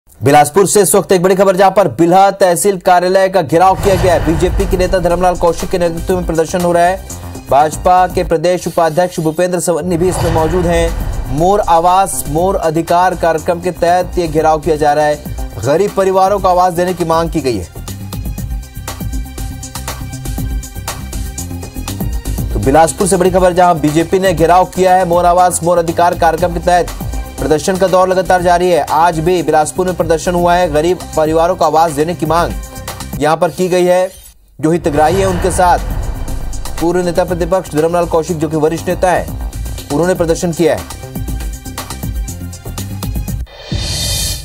BJP नेता Dharamlal Kaushik के नेतृत्व में प्रदर्शन | Bilha Tehsil Office का किया घेराव
IBC24, BJP नेता Dharamlal Kaushik के नेतृत्व में प्रदर्शन, Bilha Tehsil Office का किया घेराव